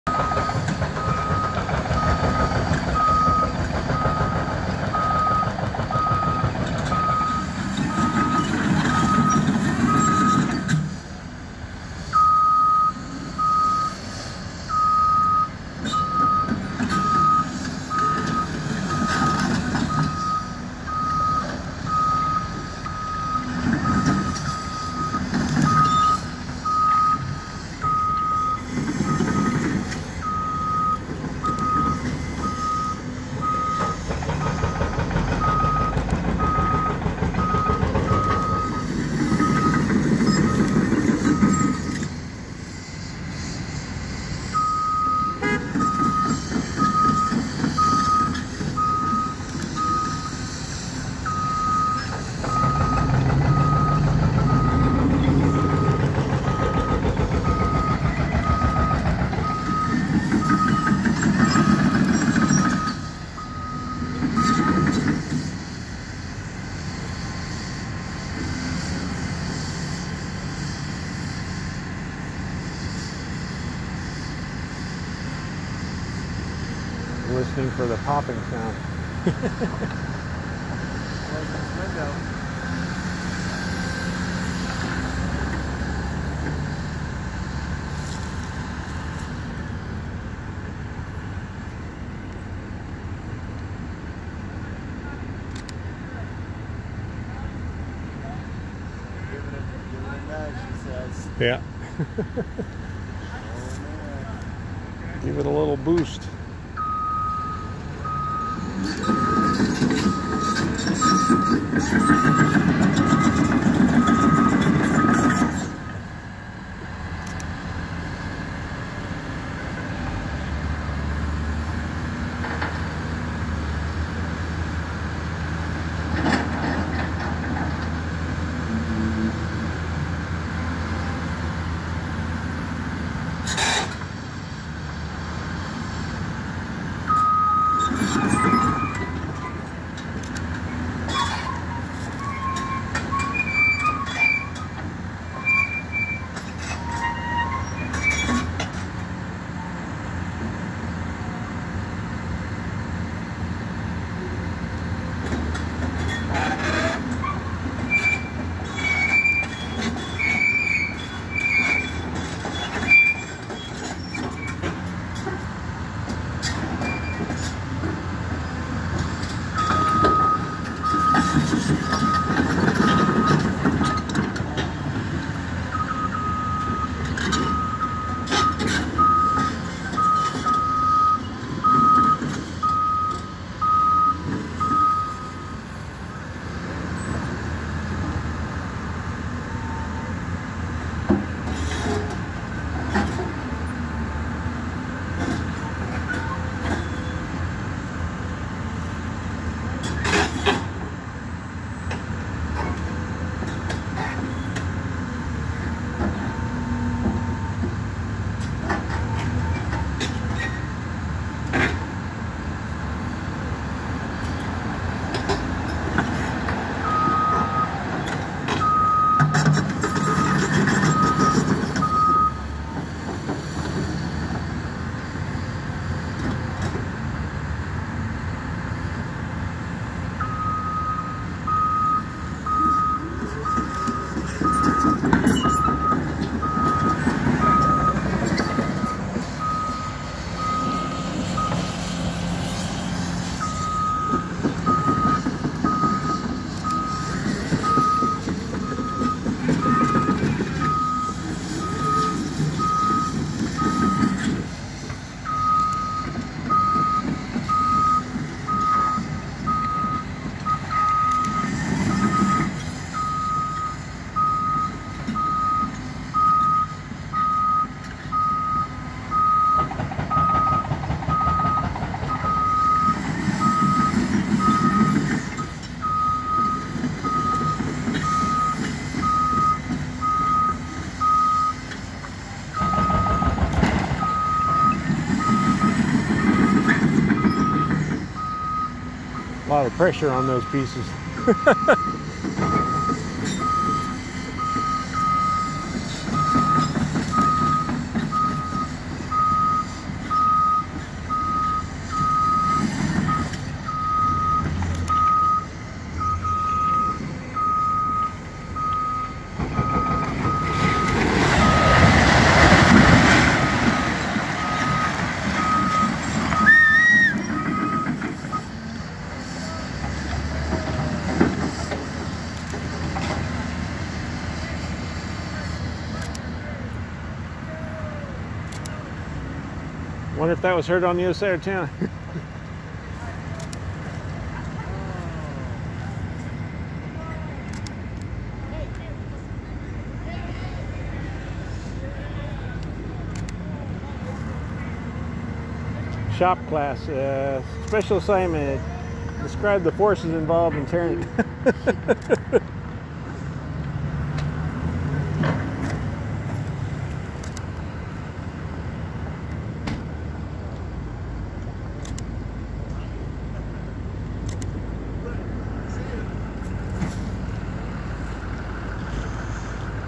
2021_12_08 Wesley United Methodist Bell Tower Fall
Wesley United Methodist Bell tower location in Ottumwa, IA.  Demolition by Zach De Carlo Company out of Des Moines, IA on December 8, 2021